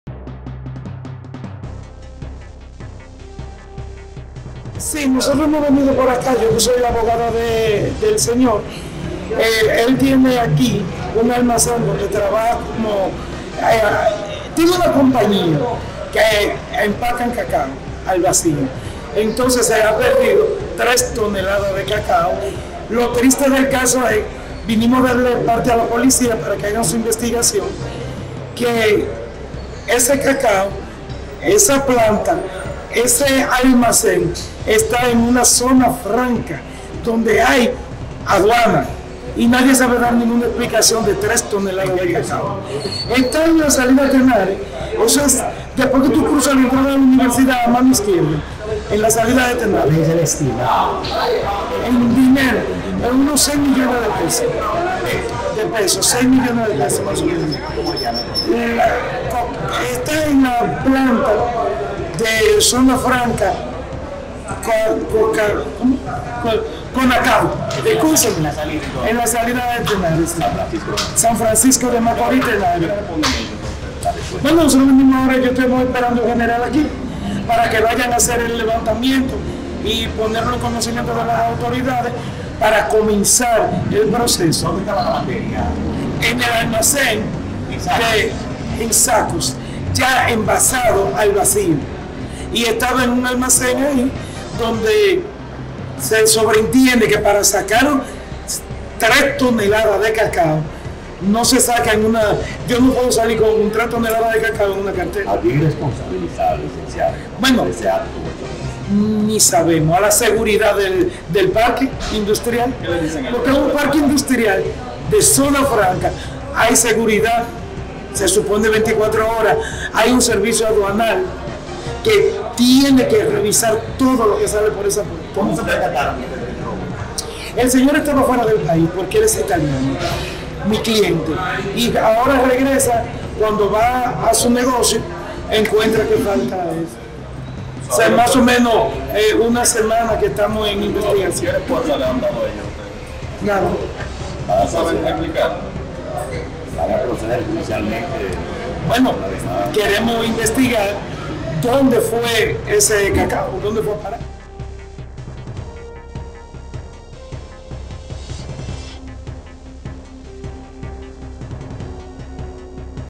Denuncian robo millonario de toneladas de cacao en SFM
San Francisco de Macorís. - Personas aún no identificadas penetraron a la sede de la Confederación Nacional de Cacaocultores Dominicanos (CONACADO), donde robaron tres toneladas de cacao empacados al vacío.

De acuerdo con las declaraciones de la abogada Irka Rodríguez, su cliente se percató del robo luego de regresar al país hace una semana y, hasta la fecha las autoridades de CONACADO no han dado respuesta sobre el paradero de las toneladas de cacao.

Ante la falta de información por parte de la organización cacaocultora, decidió dar parte a la Policía Nacional, con el fin de investigar el robo cuyas pérdidas están valorada en RD$6,000,000.00.